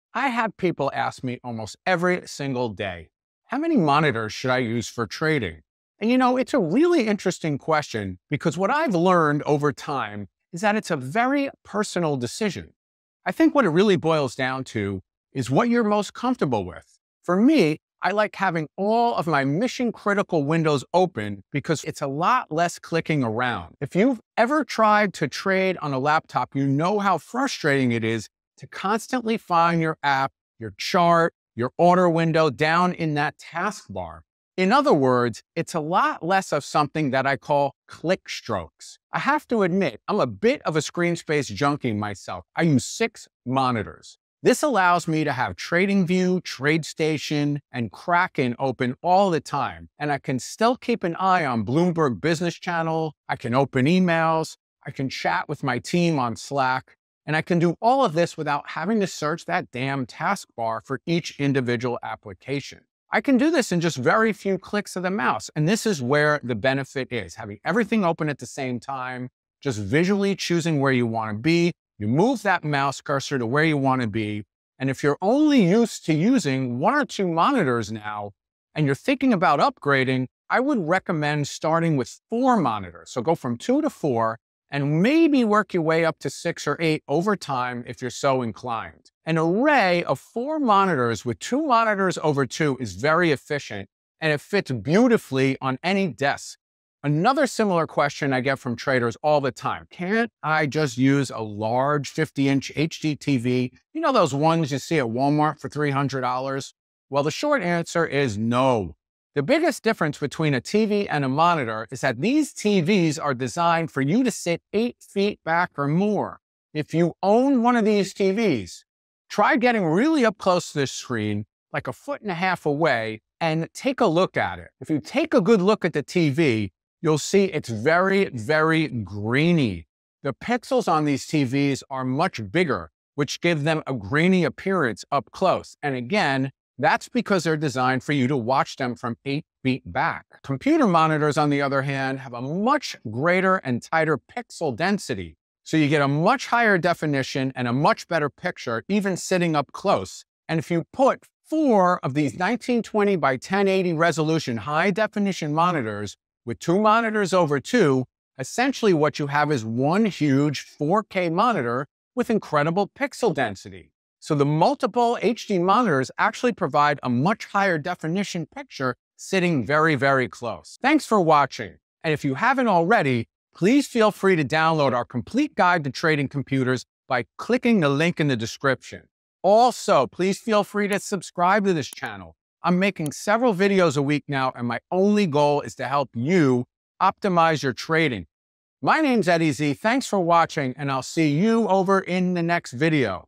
How many monitors does a day trader need?
Is your computer fast enough for trading?

Need trading help? Grab Eddie Z's Ultimate Guide to Trading Stocks

May the trend be with you,

~ Eddie Z, CEO, EZ Trading Computers